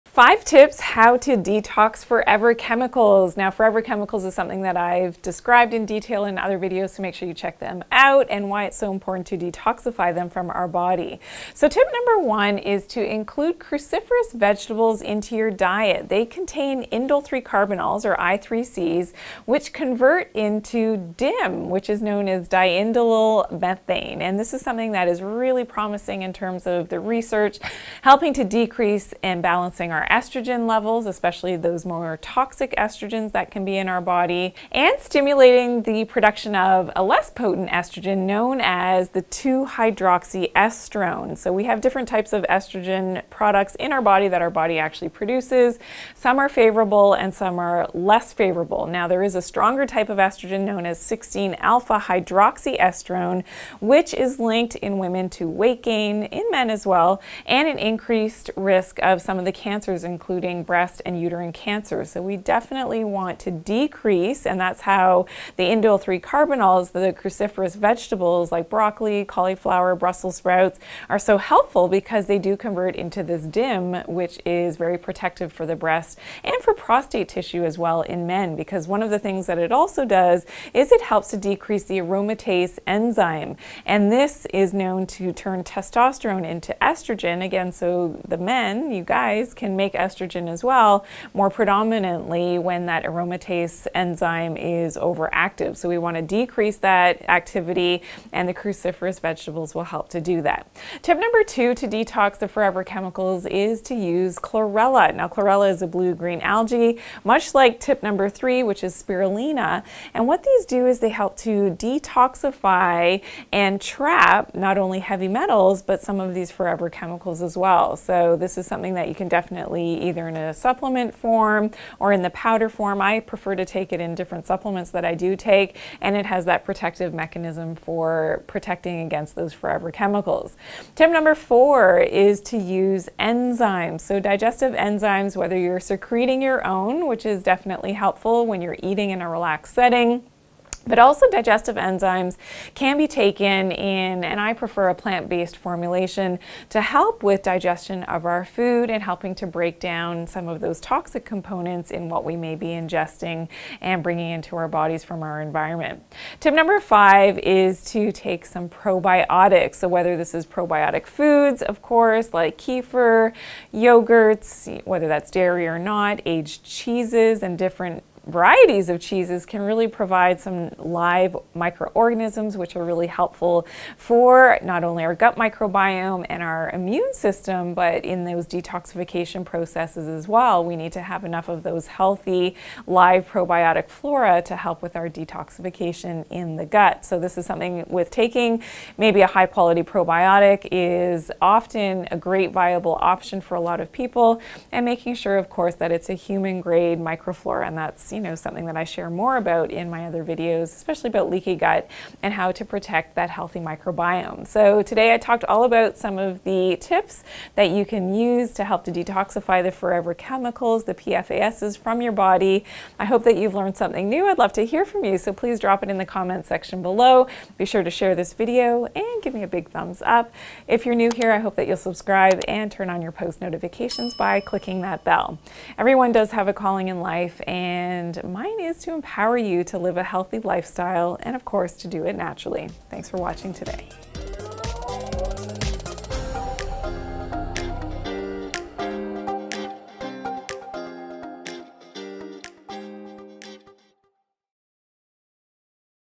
5 Tips How to Detox Forever Chemicals | Dr. J9 Live
5 Tips How to Detox Forever Chemicals | Dr. J9 Live

In this video Dr. Janine share her 5 tips how to detox forever chemicals from the body.  She talks about eating more cruciferous veggies and the benefits of I3Cs (Indole-3-Carbinols).  She explains the importance of chlorella and spirulina.  She suggests taking a digestive enzyme supplement.  Lastly, Dr. Janine explains the importance of probiotics for detoxing forever chemicals.

Video Chapters
00:00 – Intro
00:14 – Tips #1 Eat More Cruciferous Veggies
01:42 – Tip #2 Chlorella
01:49 – Tip #3 Spirulina